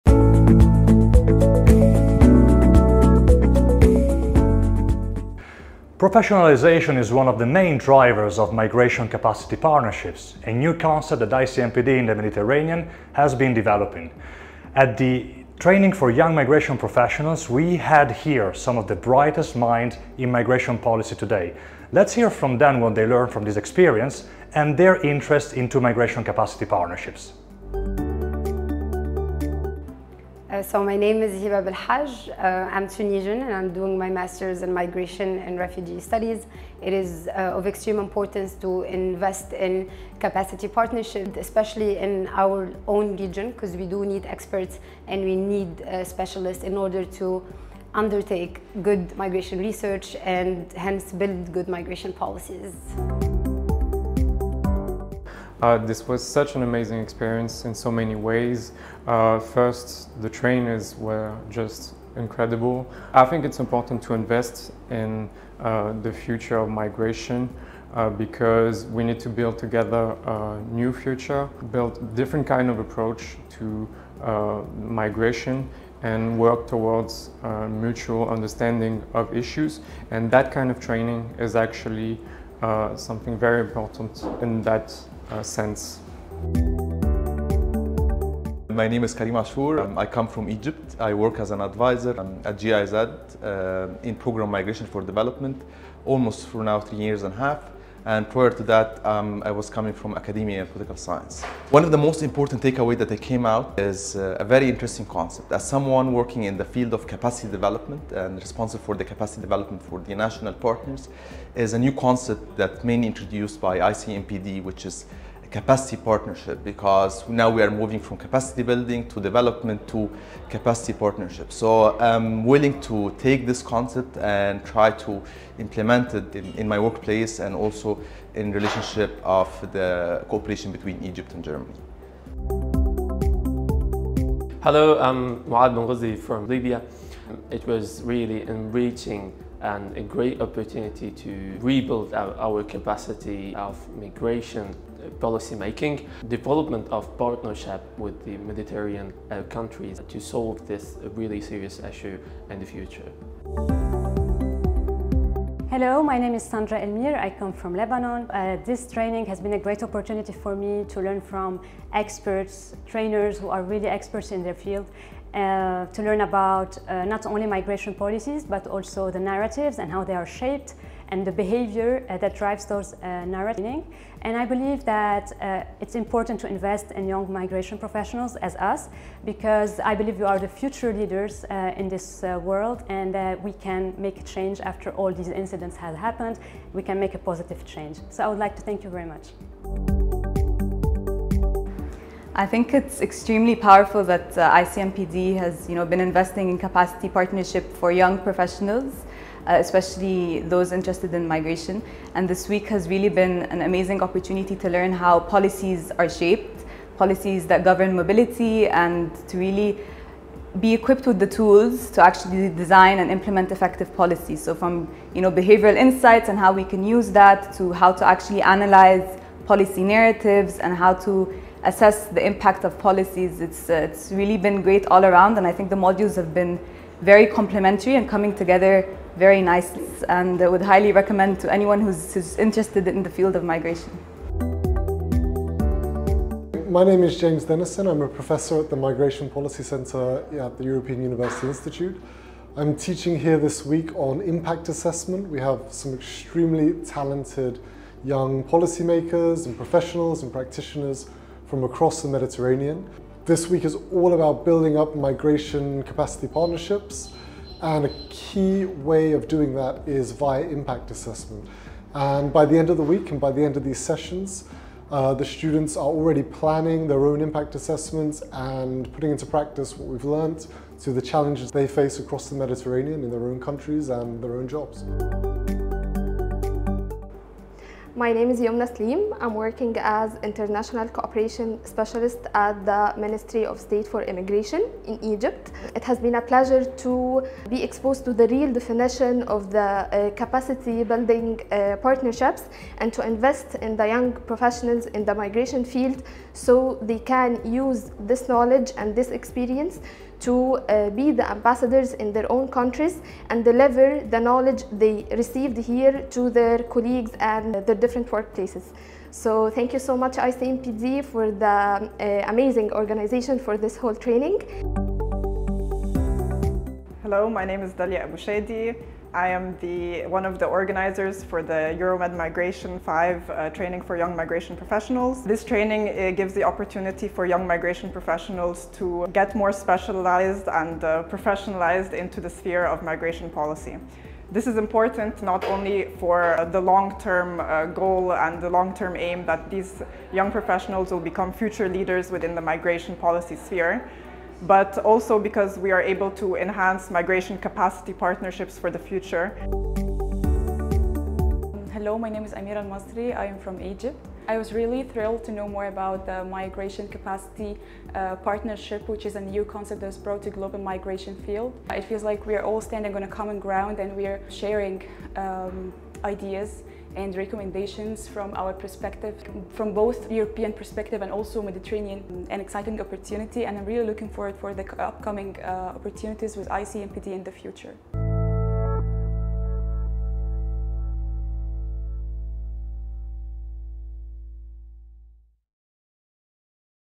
Training for Young Migration Professionals
Professionalisation is one of the drivers of MigrationCapacityPartnerships, a new concept that ICMPD in the Mediterranean has been developing.

At the Euromed Migration Training for Young MigrationProfessionals we brought together some of the brightest young minds in the field of migrationpolicy. Let’s hear from them what they learnt from this experience and their interest into MigrationCapacityPartnerships.

This training was hosted by the Training Institute on Migration Capacity Partnership for the Mediterranean